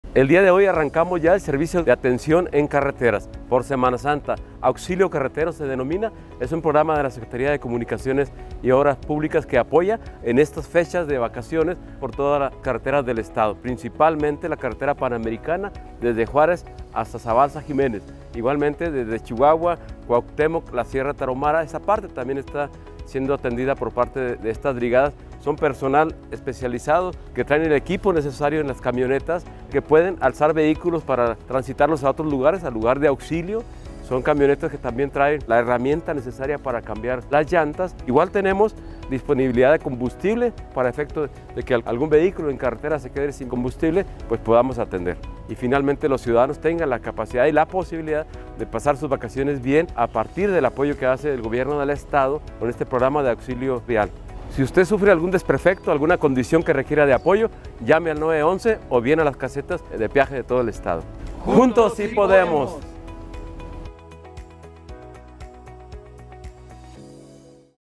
Activan Auxilio Carretero en salidas a Juárez, Cuauhtémoc, Delicias
El Gobierno del Estado de Chihuahua anunció este sábado que puso en marcha el programa "Auxilio Carretero" para esta temporada vacacional, con el objetivo de brindar apoyo a los turistas y viajeros en caso de cualquier desperfecto mecánico en su vehículo en esta Semana Santa.

El secretario de Comunicaciones y Obras Públicas, Mario Vázquez, dio el banderazo al programa, con el despliegue de cuatro volantas compuestas por camionetas Ford F-350, en las que expertos mecánicos de la SCOP estarán disponibles para auxiliar a los vacacionistas desde el sábado 1 de abril hasta el 15 de abril.